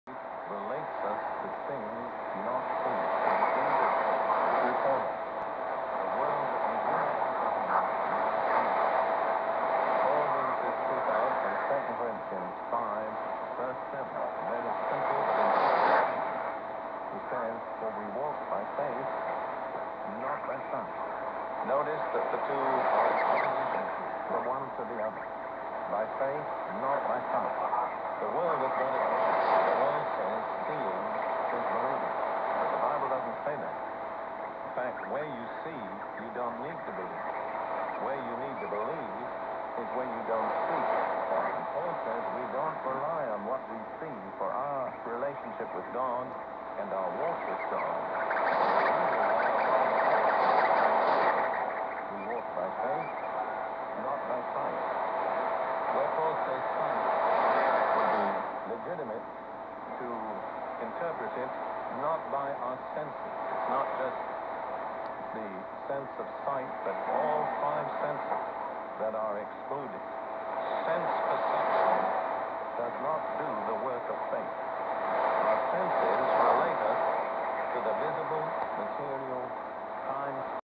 9545kHz Solomon Islands Broadcasting Corp.
Received in Yokohama, Japan at 19:13 UTC on February 10, 2014